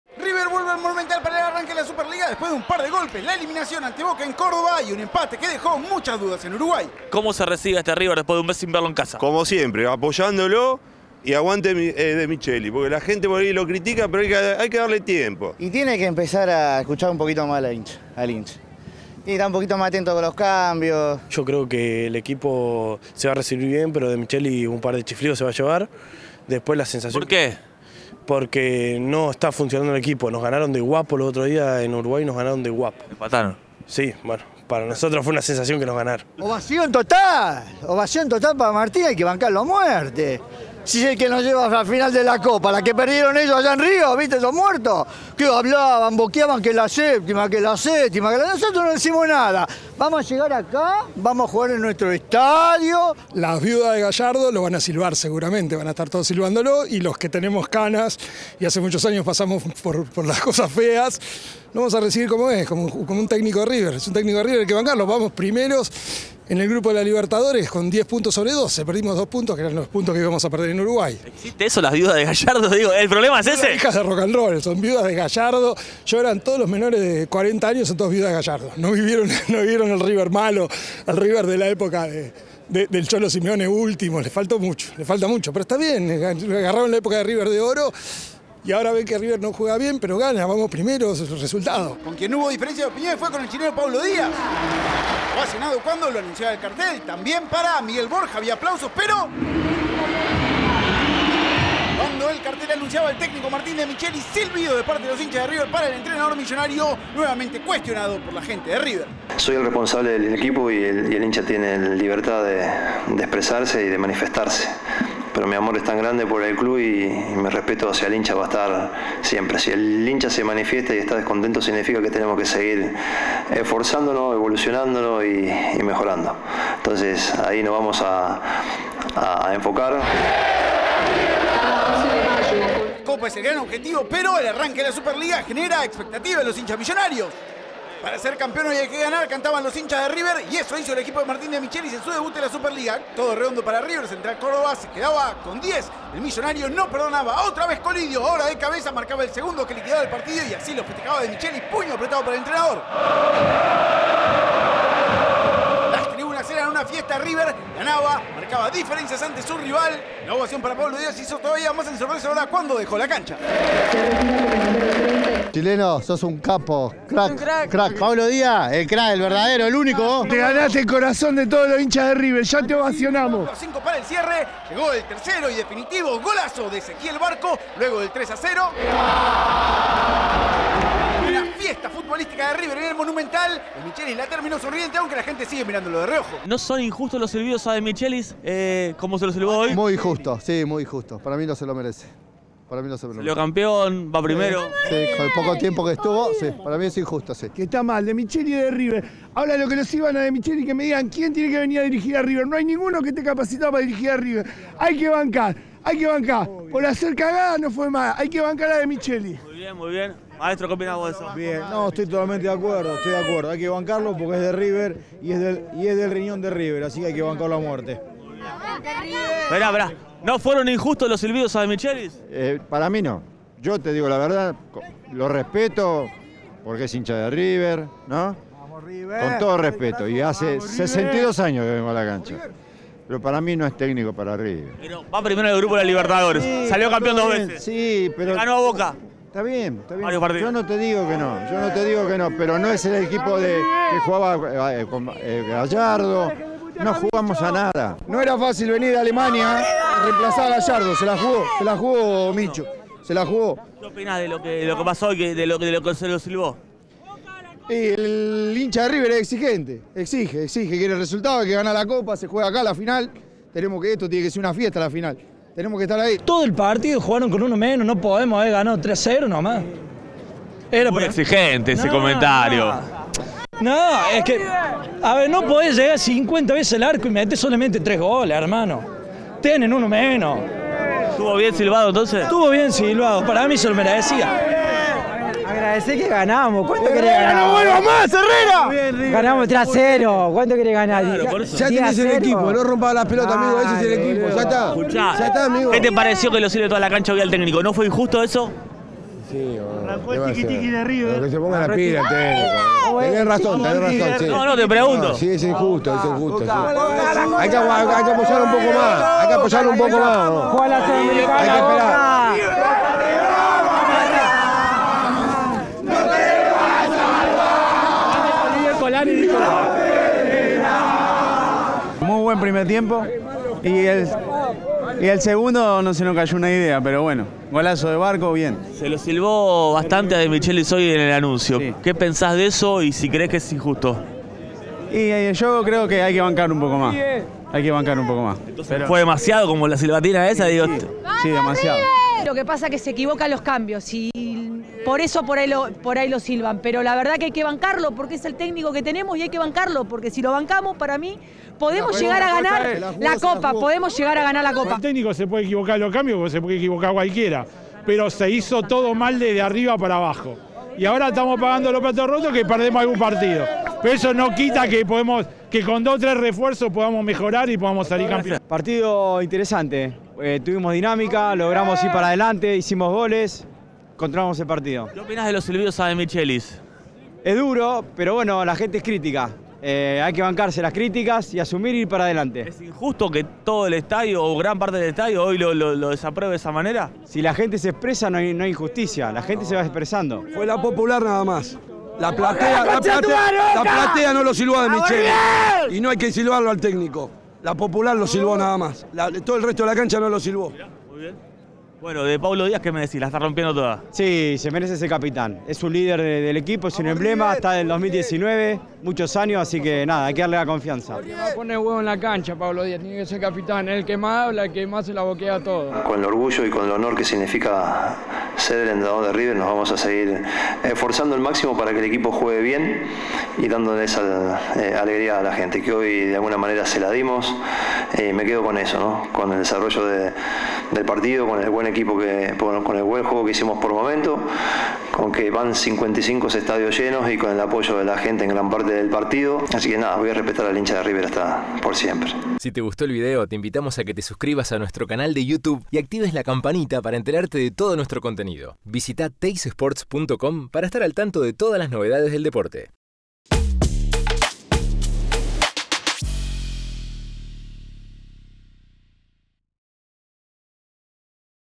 LO QUE NO VISTE DE RIVER vs CENTRAL CÓRDOBA 🔥 SILBIDOS A DEMICHELIS Y OPINIONES DIVIDIDAS DE HINCHAS
River se floreó este sábado en su debut en la nueva Liga Profesional con un 3-0 sobre Central Córdoba de Santiago del Estero en el Monumental, pero en la previa la gente millonaria dio que hablar al silbar profusamente a su entrenador Martín Demichelis. Claro, el equipo no se presentaba en su casa desde hace un mes y, al parecer, el buen andar en la Copa Libertadores no le alcanzó para borrar el cachetazo de Boca en la eliminación de la Copa LPF y reavivó alguna recriminación.

Por supuesto, en la conferencia de prensa posterior a la goleada se esperaba que Micho respondiera sobre el tema. Y el DT lo hizo sin perder la compostura: "Soy el responsable del equipo y el hincha tiene libertad de expresarse y manifestarse, mi amor es tan grande por el club que mi respeto hacia el hincha va a estar siempre. Si el hincha se manifiesta y está descontento significa que tenemos que seguir esforzándonos, evolucionando y mejorando. Ahí nos vamos a enfocar con el orgullo y el honor que significa ser entrenador de River, vamos a seguir esforzándonos al máximo para que el equipo juegue bien y dándole alegría al a gente, que hoy de alguna manera se la dimos".

"Me quedo con eso, el desarrollo del partido, el buen jugo que hicimos por momentos, van 55 estadios llenos con el apoyo de la gente en gran parte del partido. Me alegra mucho que haya llegado el tercer gol porque creo que faltaba eso para terminar de ponernos a festejar, así que voy a respetar al hincha de River por siempre", aseguró el DT.

El análisis de una noche redonda

Acerca del trámite del encuentro, el técnico valoró: "Hicimos un muy buen primer tiempo donde intentamos ser muy intensos desde la presión y por momentos lo logramos ante un rival con entrenador nuevo desde hace dos o tres semanas con ideas muy valientes, vinieron a jugar desde la posesión a intentar superarnos en cada una de nuestras presiones dándole un sentido al pase y por momentos nos tuvimos que ir acomodando pero estuvimos muy compactos, intensos, administramos muy bien la pelota y generamos muchas situaciones, con la expulsión siguió el desarrollo de ese primer tiempo y llegó el 2-0".

Demichelis continuó: "En el segundo administramos el partido y lamentablemente el gol llegó tarde para levantar a la gente pero de todas maneras llegó, me alegra muchísimo que encima lo haya hecho Esequiel (Barco) que lo venía buscando, había tenido situaciones en el primer tiempo y su figura, el arquero, lo había impedido. Contento por arrancar el torneo con tres goles, valla en cero, habiéndole dado posibilidad a algunos chicos que no venían jugando y los que entraron desde el banco también. Barco el año pasado tuvo un gran año, todavía no había pedido convertir y quedó en evidencia que estábamos necesitando el gol por el desahogo de todos".

"Estamos en un proceso todavía de evolución, si bien venimos repitiendo el sistema táctico en los últimos partidos independientemente de los nombres estamos mucho mejor en eso, dentro del mismo sistema encontrar las mejores opciones. Hoy con cinco cambios hicimos en líneas generales un buen partido porque necesitamos de todos para competir de acá hasta el final. Es muy esperanzador, nos quedan dos partidos de la fase de grupos, el otro martes la Copa Argentina que quedó reflejado que no es fácil y después este torneo donde nos quedan 26 finales más, queremos ser protagonistas hasta diciembre", completó más tarde.